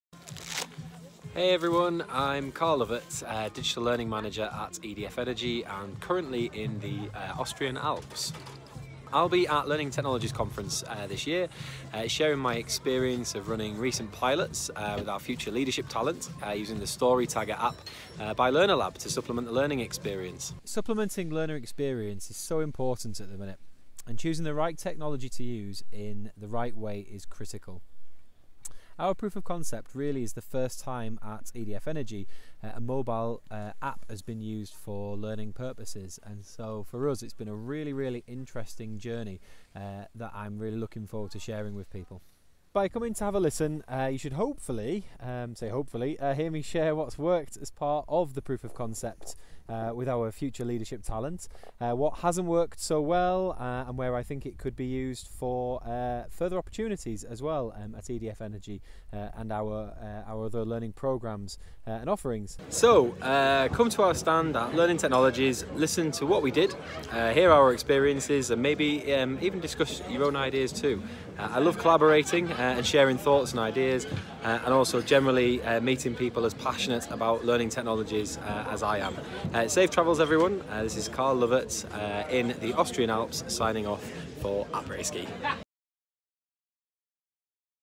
Carl Lovett at Learning Technologies 2019 - Preview
Find out how EDF Energy is pioneering user-generated mobile video to help its future leaders transfer learning with a more social, reflective and innovative experience.

Carl Lovett, Digital Learning Manager at EDF Energy, previews his seminar at Learning Technologies 2019.

The EDF Energy story: Extending the classroom experience with learner-generated video
14th February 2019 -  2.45pm - Theatre 11.